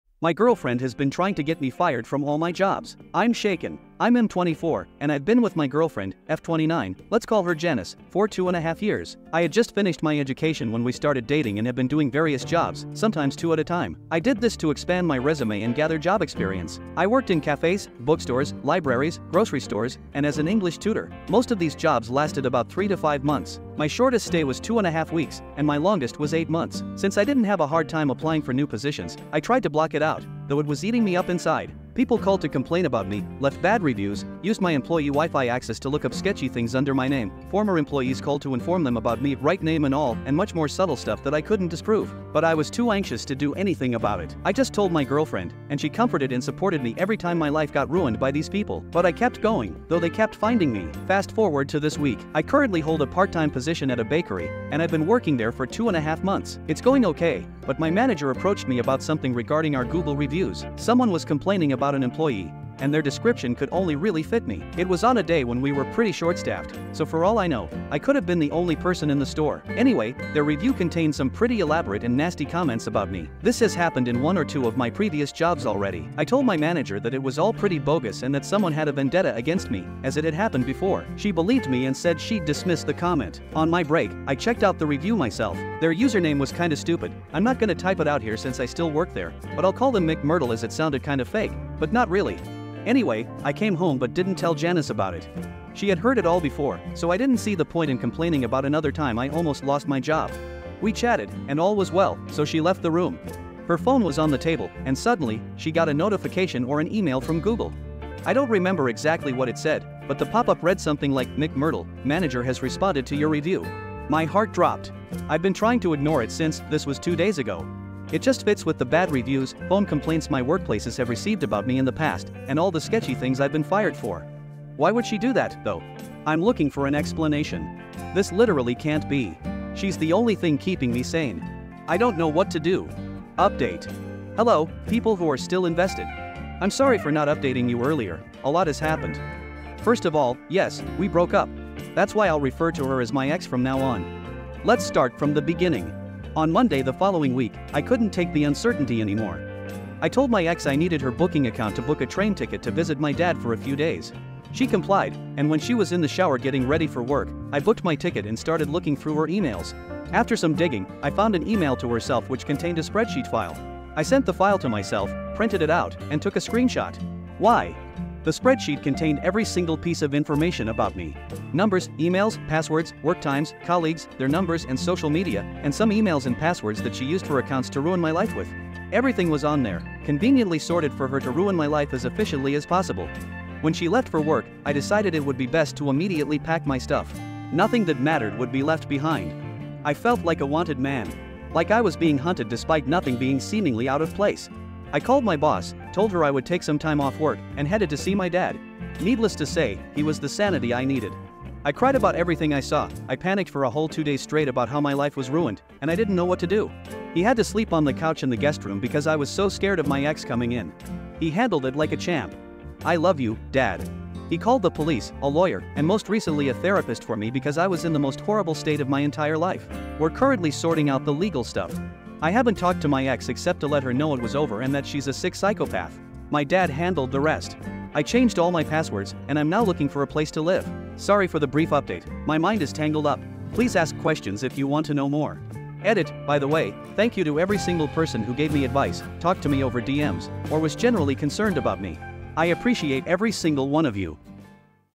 My Girlfriend Has Been Trying to Get Me Fired From All My Jobs. I'm Shaken.
Title: My Girlfriend Has Been Trying to Get Me Fired From All My Jobs. I'm Shaken.

I'm M24, and I've been with GF (F29), let's call her Janice, for 2 and a half years. I just finished my education when we started dating and I have been doing various jobs sometimes two at a time.

*Subreddit Safari does not own all the materials in the video but adheres to copyright laws and employs Fair-Use law appropriately.